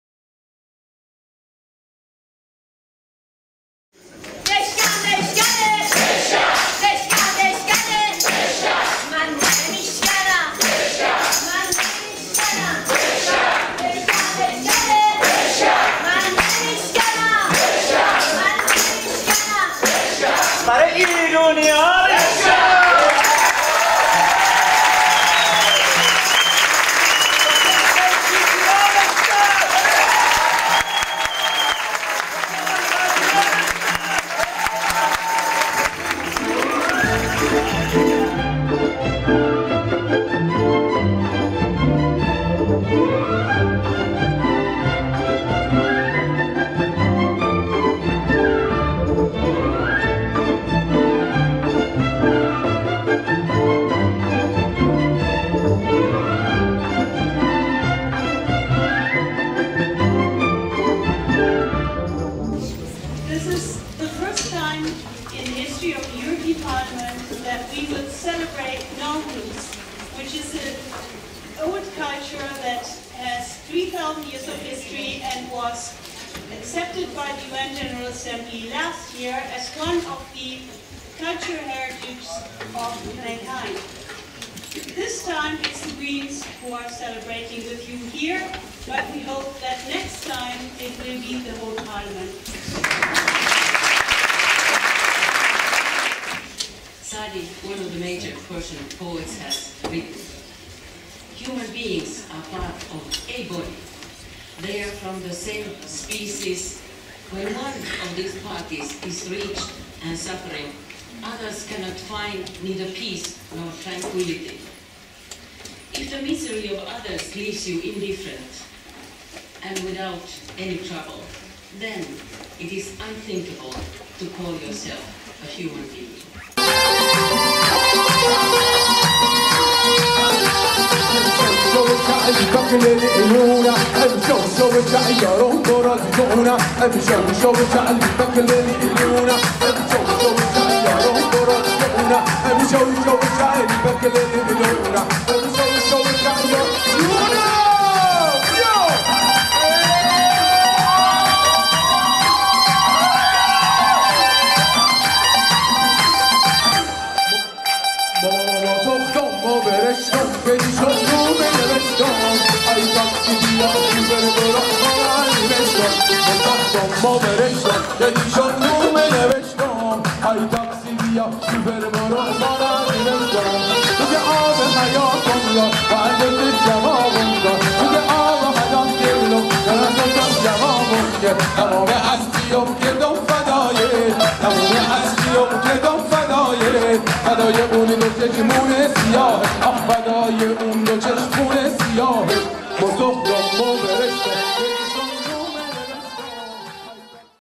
First Celebration of Persian New Year at European Parliament, Video: Radio Zamaneh, 2012
نخستین جشن گرامیداشت نوروز در پارلمان اروپا - مارس 2012 - ویدئو و ادیت: پژمان اکبرزاده / رادیو زمانه / The first celebration of Nowruz (Persian New Year) at European Parliament in Brussels, March 2012, Camera & Edit: Pejman Akbarzadeh / Radio Zamaneh